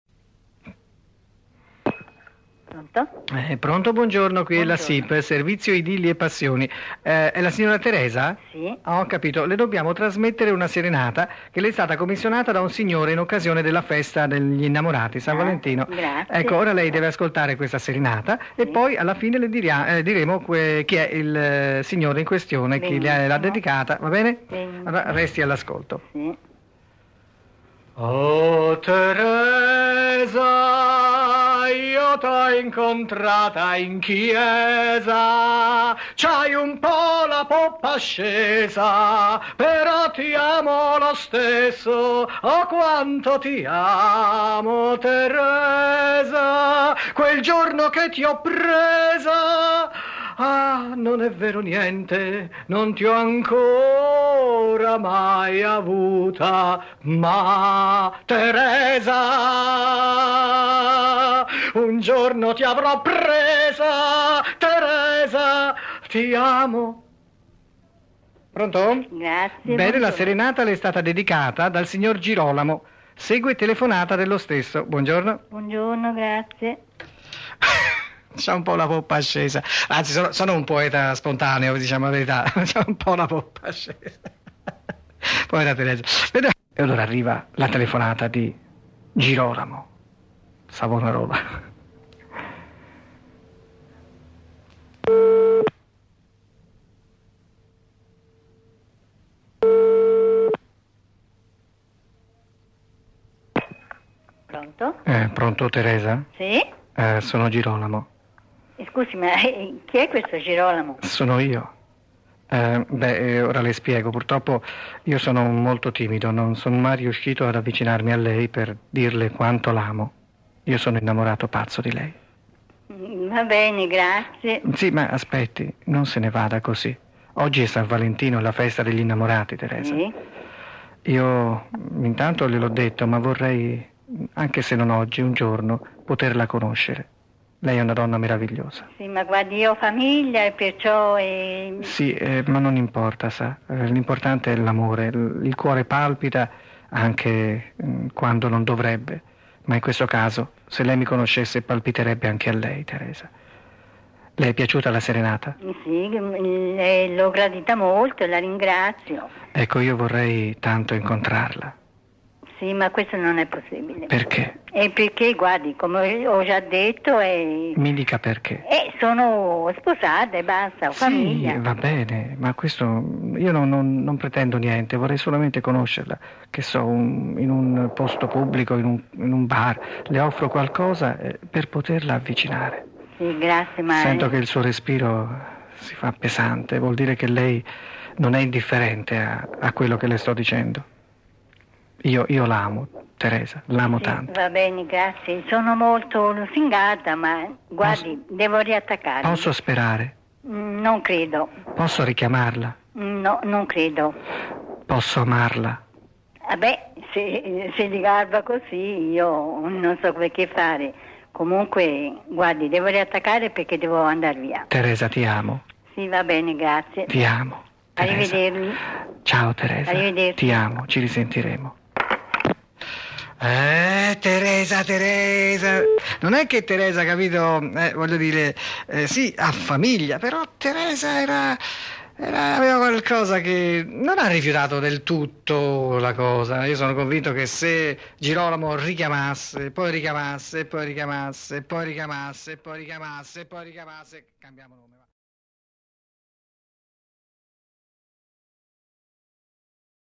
#51 Sondazzo Story - SIP servizio idillii e passioni (serenatiche)
Nuove esilaranti telefonate del Sondazzo del "G" risalenti ai primi anni 90.

Un ringraziamento speciale a Massimo di Scandicci per le cassette.